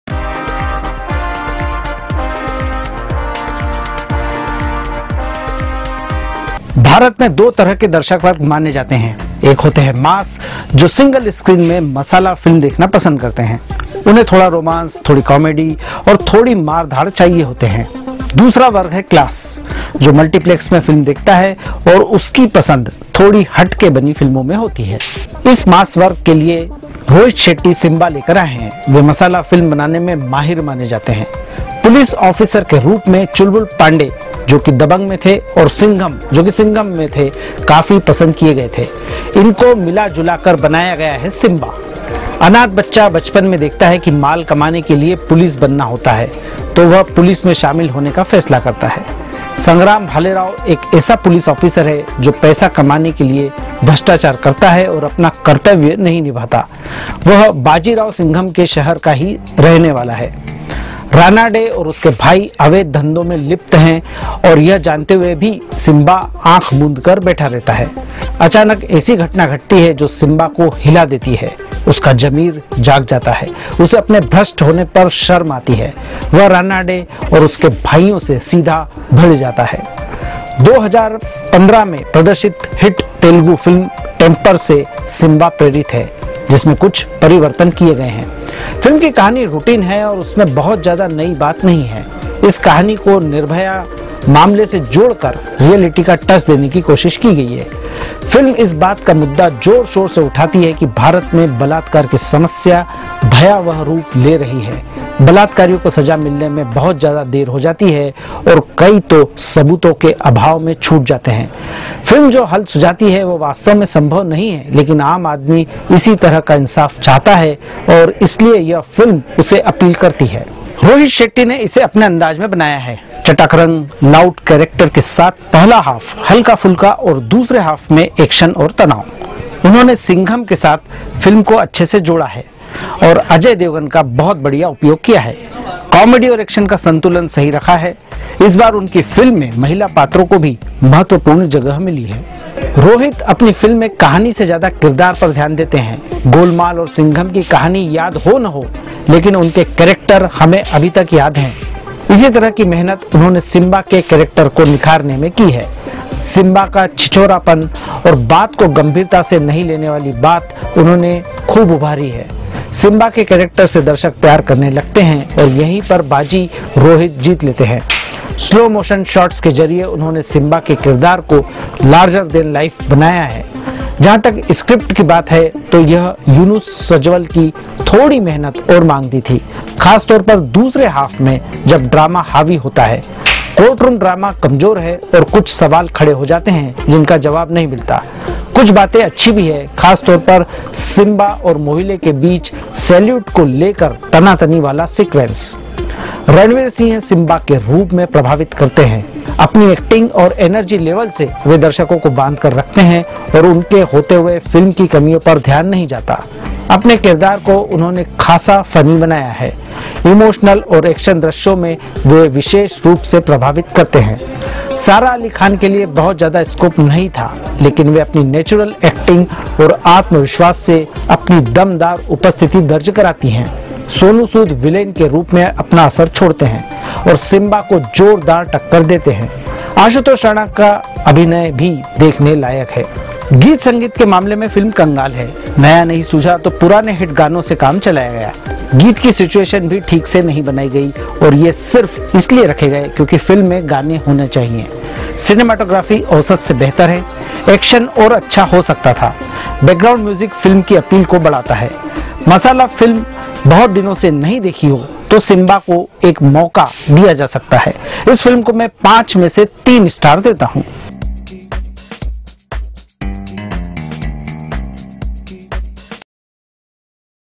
Simmba : Movie Review || सिम्बा : फिल्म समीक्षा
रोहित शेट्टी सिम्बा लेकर आए हैं। वे मसाला फिल्म बनाने में माहिर माने जाते हैं। पुलिस ऑफिसर के रूप में चुलबुल पांडे (दबंग) और सिंघम (सिंघम) काफी पसंद किए गए थे। इनको मिला-जुलाकर बनाया गया है सिम्बा।